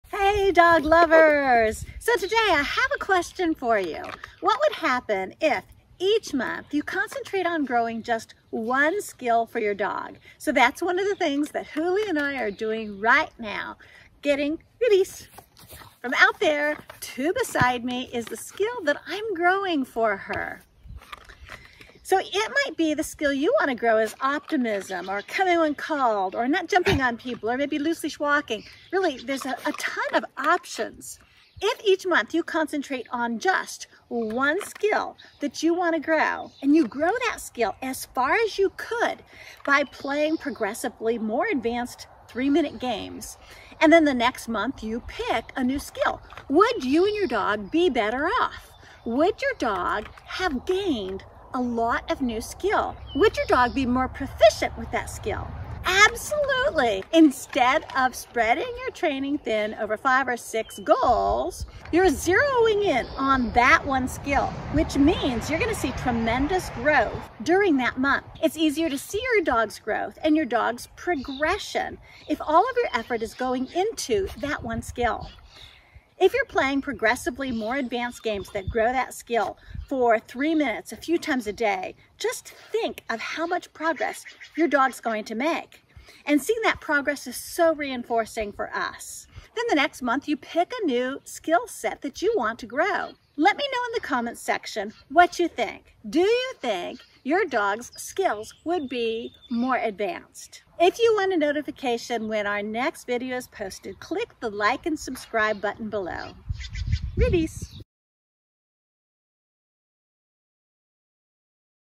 Fast Track your training by focusing on just 1 skill per month
What would happen if, each month, you concentrated on growing just 1 skill for your dog?

It might be:
Optimism
Coming when called
Not jumping on people
Reactivity
Loose Leash Walking
Really, there is a huge variety of options.

If you zero in on one skill and you grow that skill as far as you could by playing progressively more advanced 3-minute games your dog will be further along because you're not diluting your training time by covering more than 1 thing.

You're going to fast-track that skill.

Then the next month, you can pick a different skill to zero in on